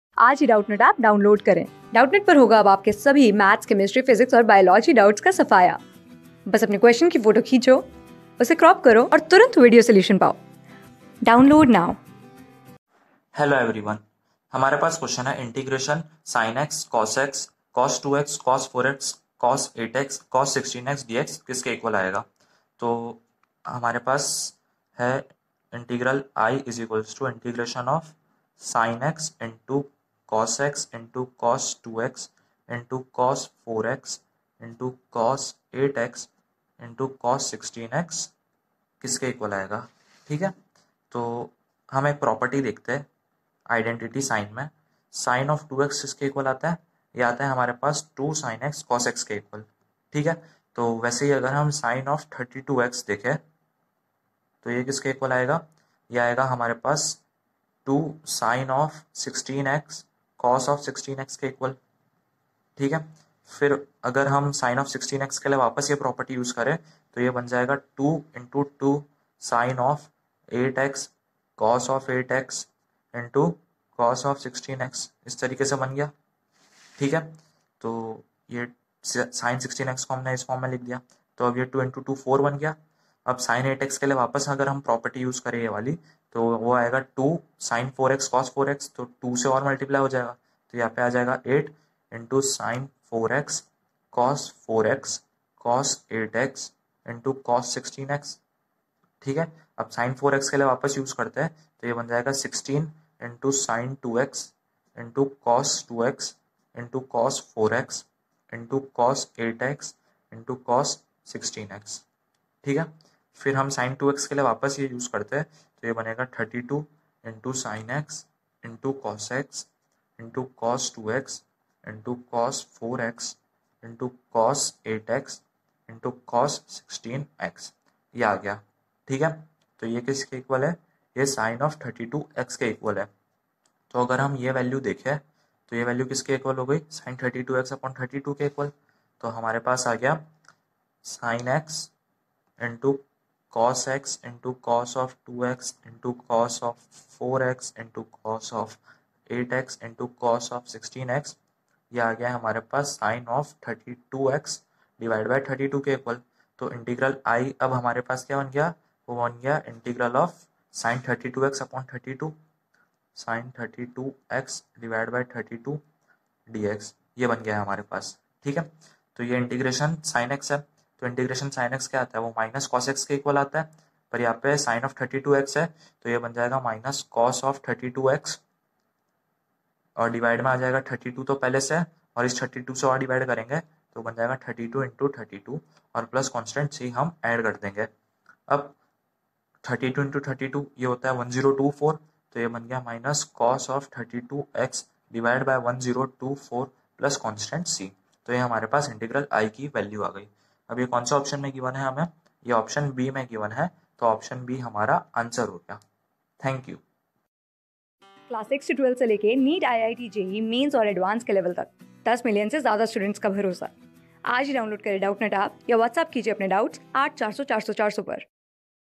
intsinx.cosx.cos2x.cos4x.cos8x.cos16xdx is equal to | CLASS 12 | INDEFINITE INTEGRATION | MATH...
intsinx.cosx.cos2x.cos4x.cos8x.cos16xdx  is equal to
Class: 12
Subject: MATHS
Chapter: INDEFINITE INTEGRATION
Board:IIT JEE

👉 Have Any Query? Ask Us.